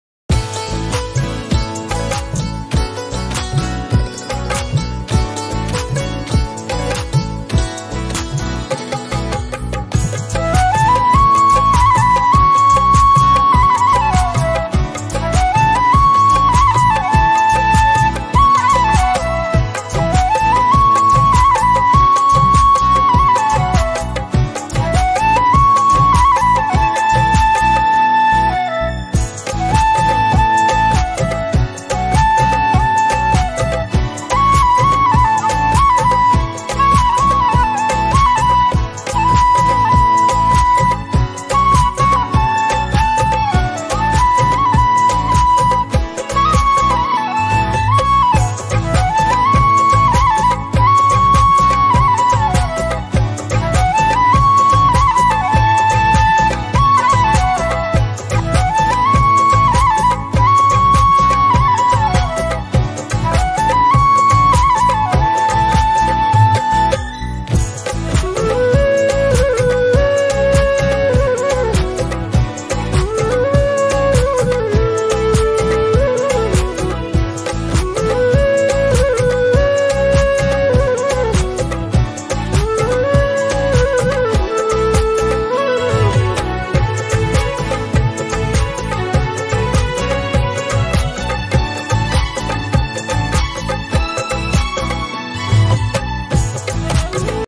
Bina pyaj lahsun ke banae karela ki chatpati sabji 😋
Bina pyaj lahsun ke banae karele ki sabji kam samay mein bankar jhatpat se taiyar ho jayegi healthy recipe